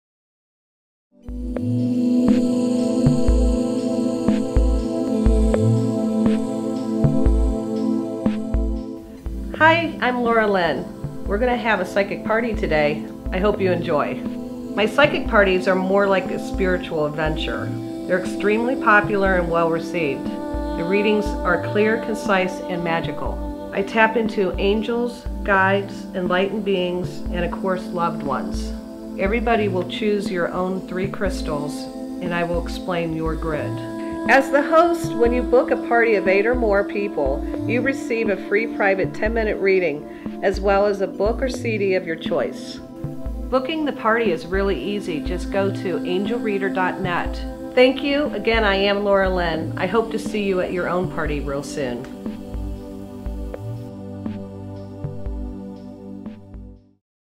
Angel Reader Laura Lyn Psychic party video.
Angel Reader Laura Lyn holds psychic parties all over NE Ohio. You can book your own party by going